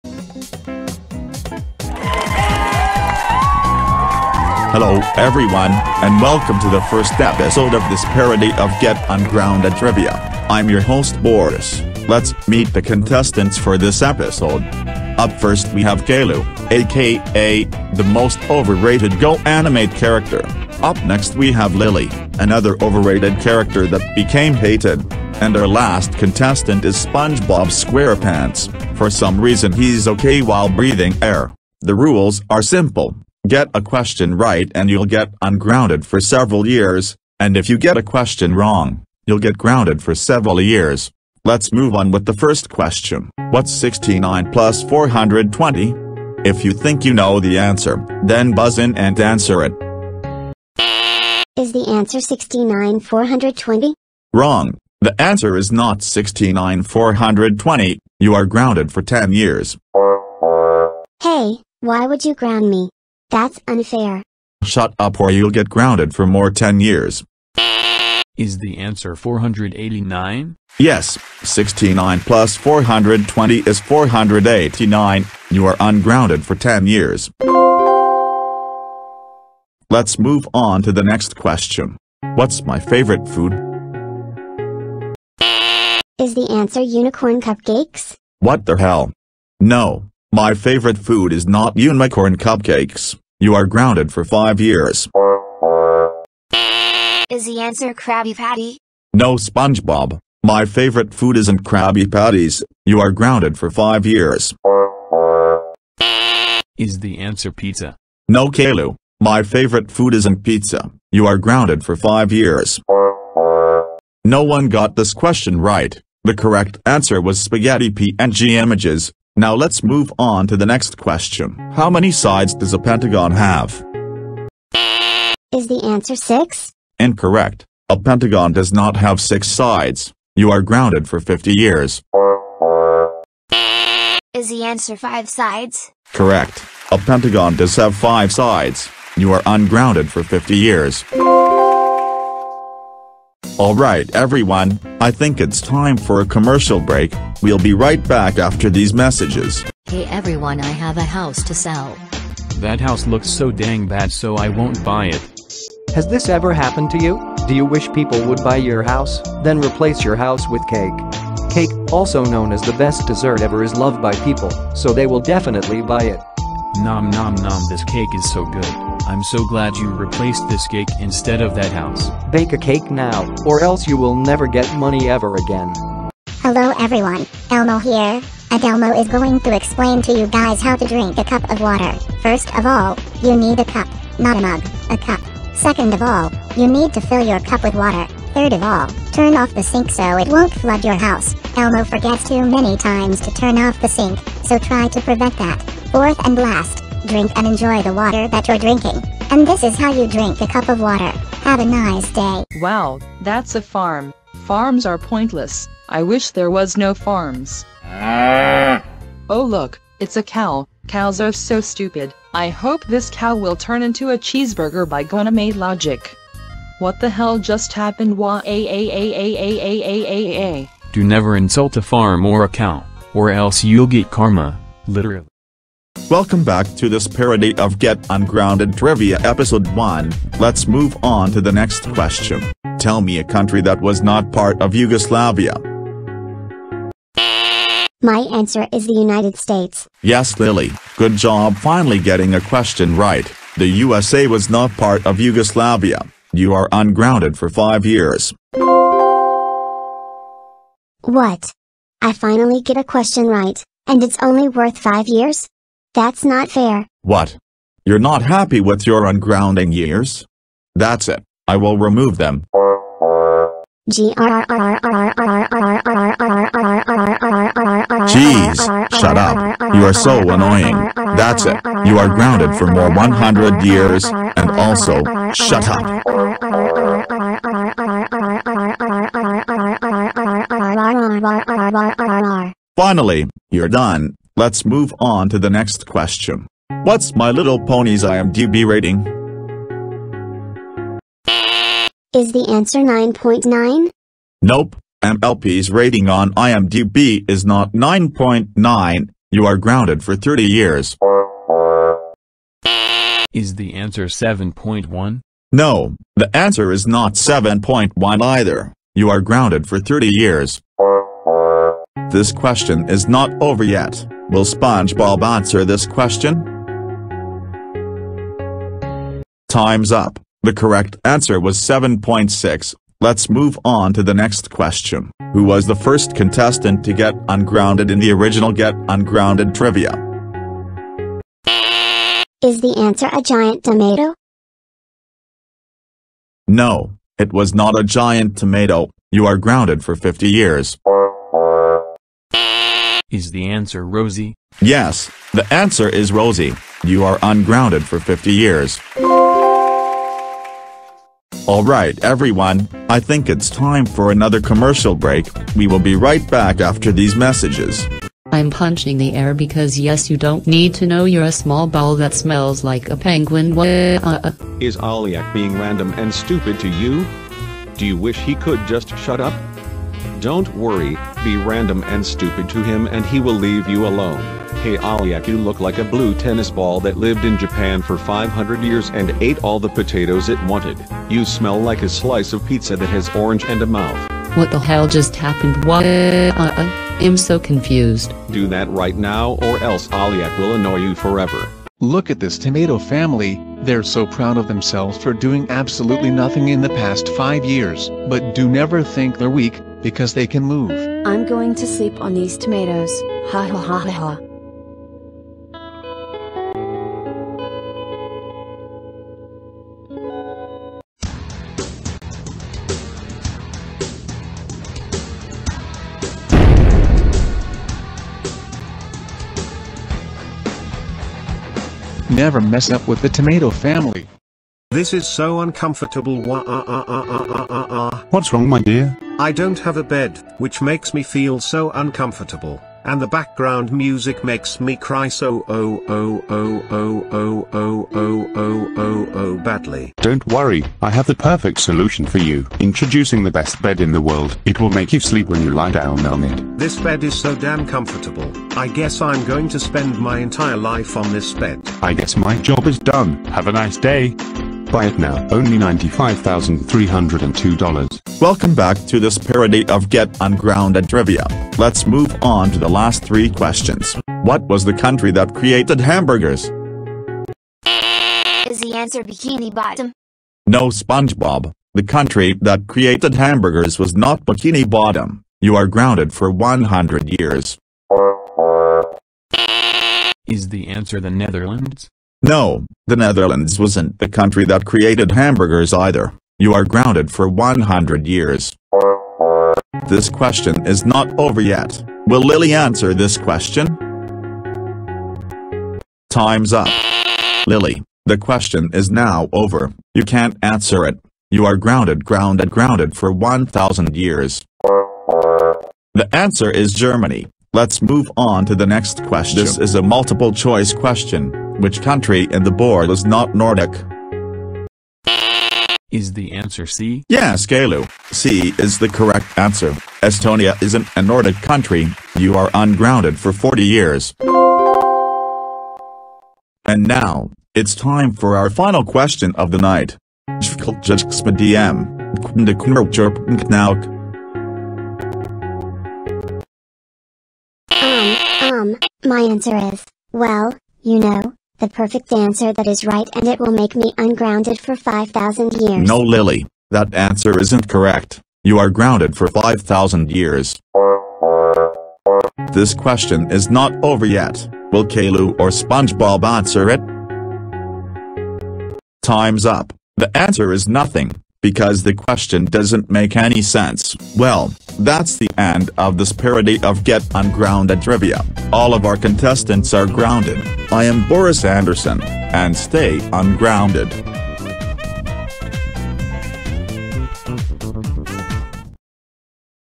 Parody of Get Ungrounded Trivia Episode 1
Caillou, Lily and Spongebob will compete in a gameshow to be ungrounded, will anyone get ungrounded?

Inspired by Pappy Animate 2012